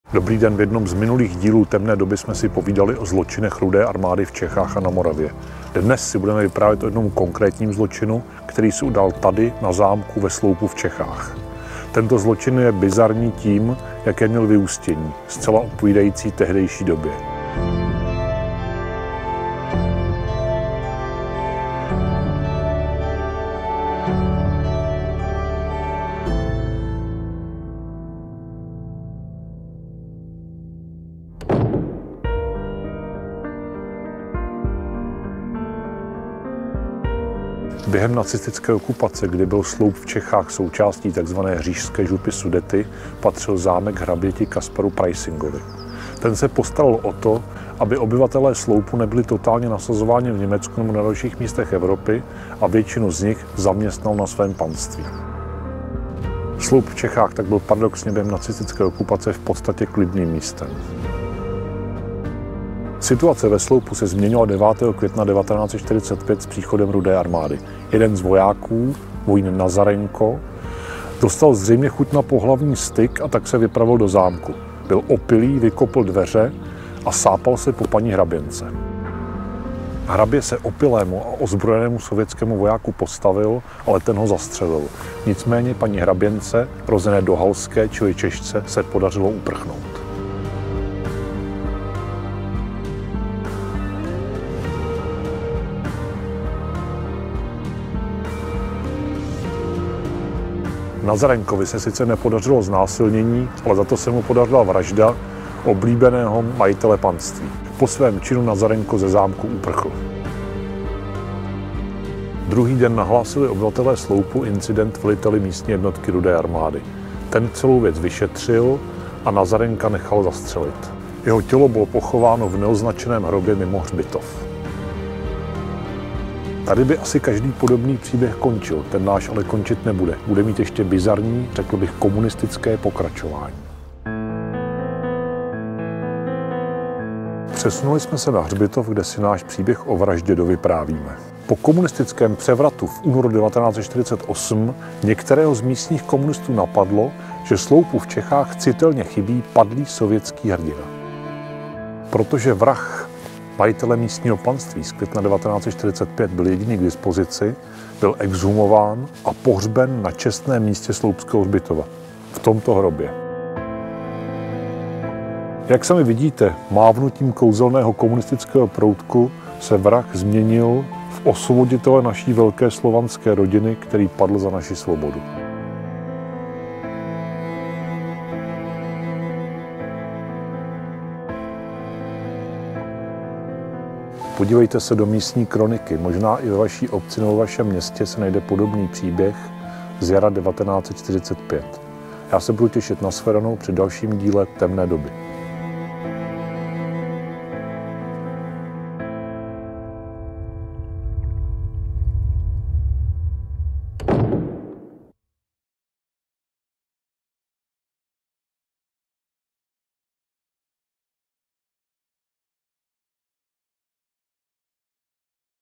Jak udělal komunistický režim z vraha sovětského hrdinu a osvoboditele
Už jsme zde mluvili o zločinech Rudé armády po příchodu na české území. Dnes se zaměříme na jeden konkrétní zločin, který se stal ve Sloupu v Čechách, kde panoval během okupace relativní klid. O ten se zasloužil i hrabě Johann Kaspar Graf von Preysing-Lichtenegg-Moos, když zařídili, aby lidé nemuseli bý totálně nasazováni, a zaměstnal je na svém panství. Situace se změnila 9. května 1945 s příchodem Rudé armády. Jeden ruský voják jménem Nazarenko se vypravil do zámku, kde se pokusil znásilnit hraběnku – ta naštěstí utekla. Johann Kaspar Graf hraběnku bránil a voják ho zabil. Po nahlášení incidentu byl pak voják zastřelen a bezejmenně pochován v masovém hrobě. To však nemělo trvat dlouho...